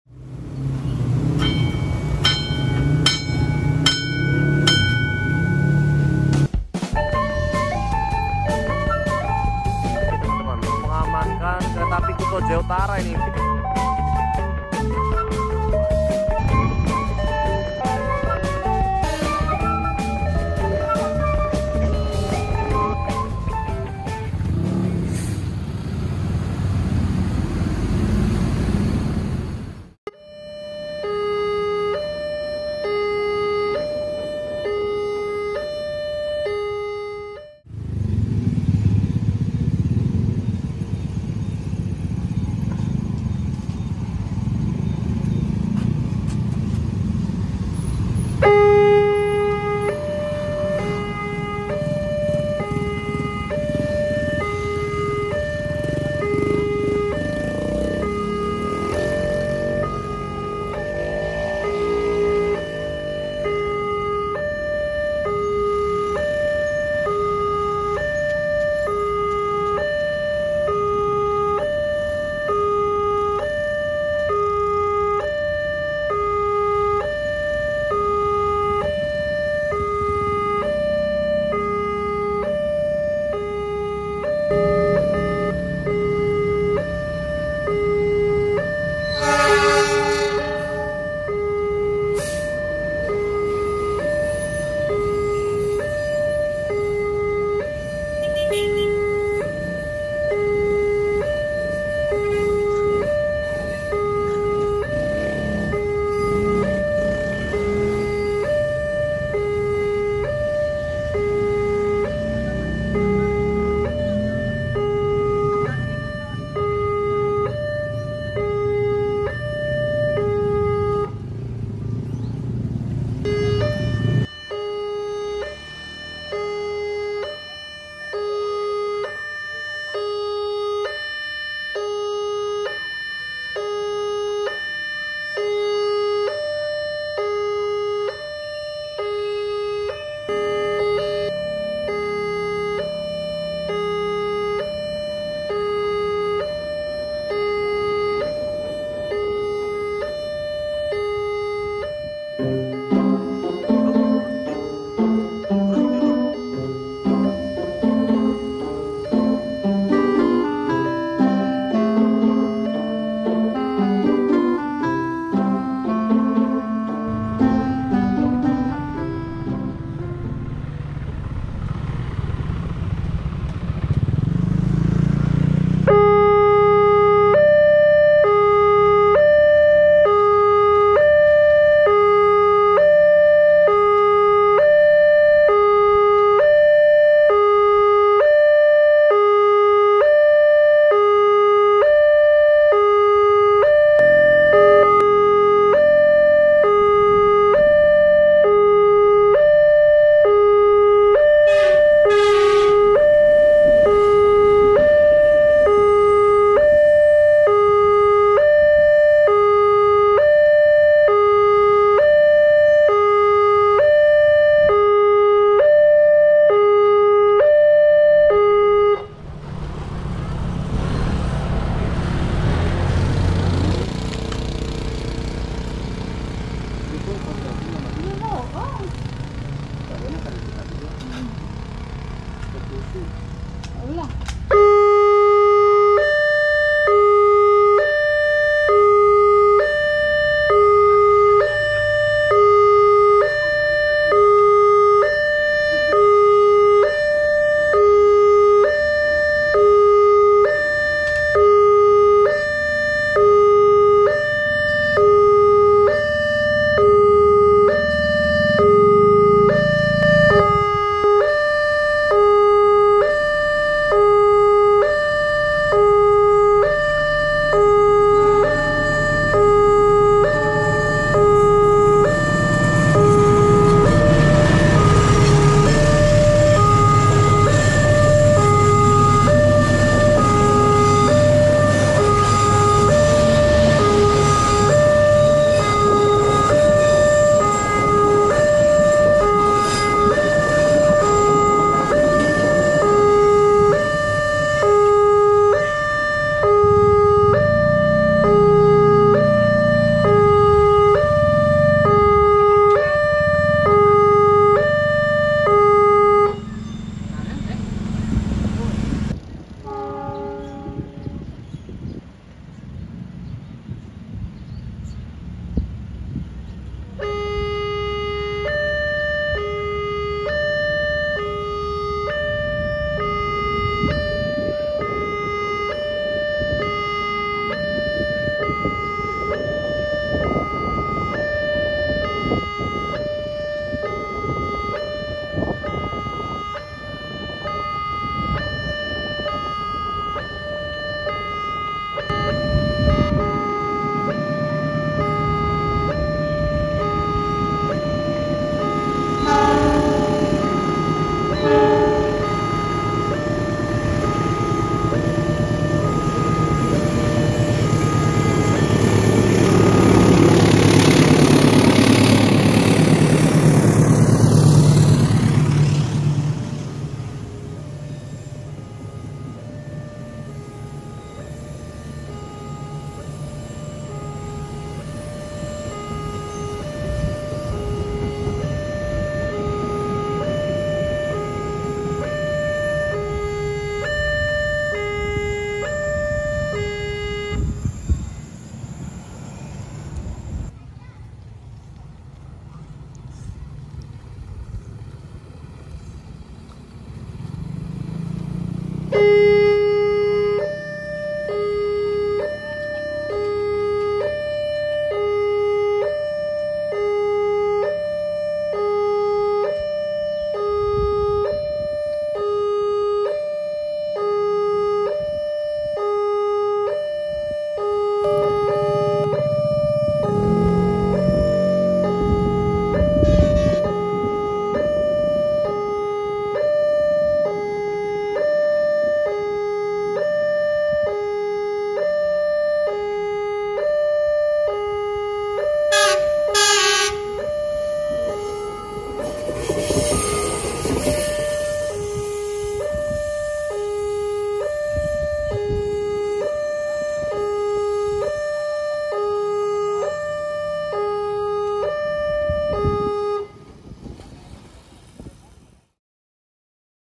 KEJADIAN UNIK di perlintasan Unik Kereta Api Cigombong Bogor, Jawa Barat
Untuk konten sebelumnya :
Disini saya hanya iseng iseng saja berkunjung kembali ke perlintasan KA ini, namun malah mendapatkan kejadian unik nih lur, yaitu saat asik merekam palang pintu menutup, yang lewat ternyata lori mobil lur, namun saat ingin melintas deket posisi saya, malah berhenti😭😅 saya kira teh saya mau ditegor, taunya hanya menurunkan pegawai yang lagi on board hehehe.